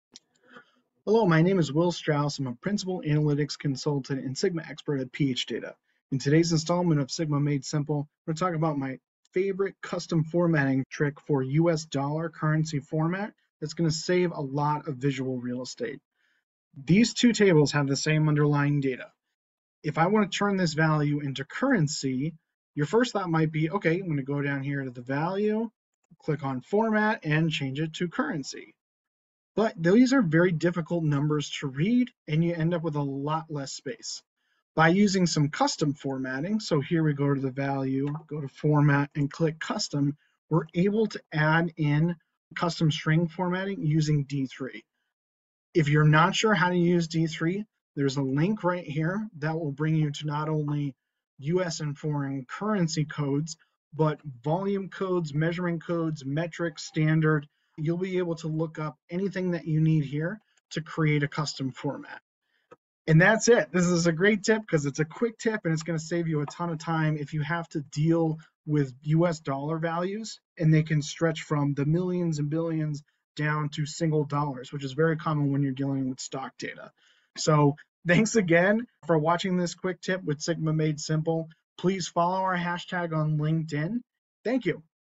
Custom $ Format Trick in Sigma in Sigma Computing
In this episode of Sigma Made Simple, Will Strouse, a principal analytics consultant and Sigma expert at phData, shares his favorite custom formatting trick for US dollar currency format. By using custom formatting with D3, Will demonstrates how to make currency values more readable and save visual space. This quick tip is especially useful for dealing with US dollar values in a wide range, from millions to single dollars, commonly encountered in stock data analysis.

To learn more about Sigma Computing, including tutorials, how-to guides, and more,